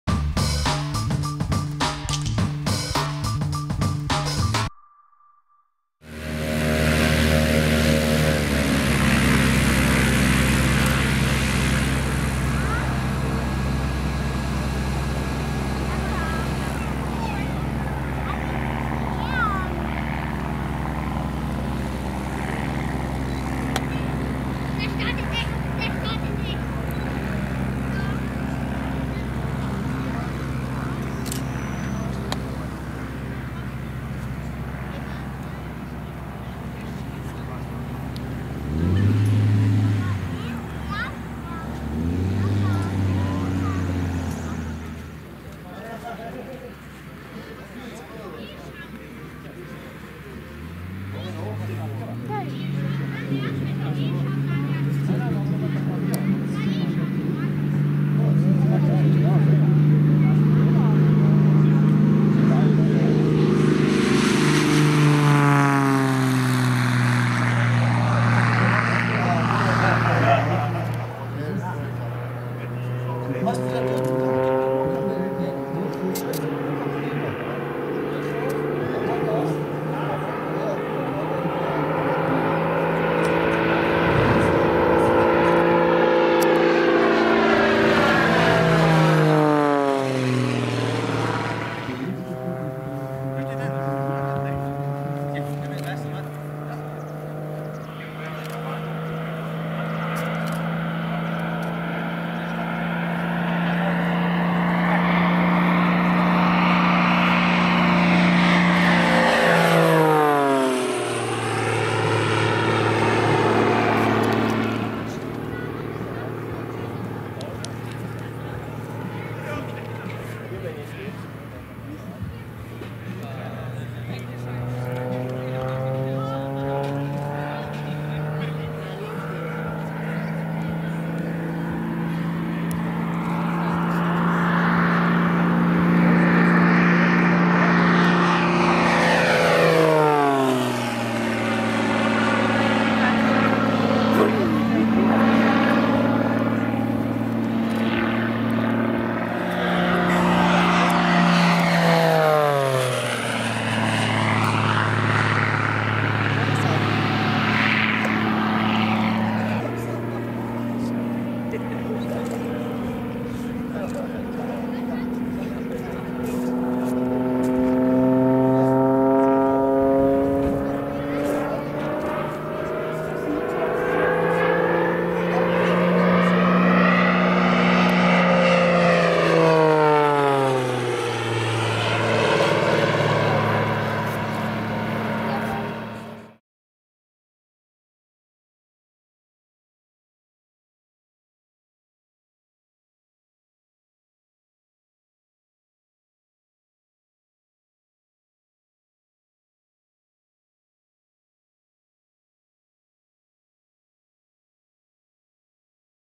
Pitts S-1 Special smoke on display at Airfield Nötsch | OE-CSW
Flugtage 2019 at Airfield Nötsch
Flugtage 2019 am Flugplatz Nötsch

Airfield Nötsch | LOKN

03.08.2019

Pitts S-1 Special
OE-CSW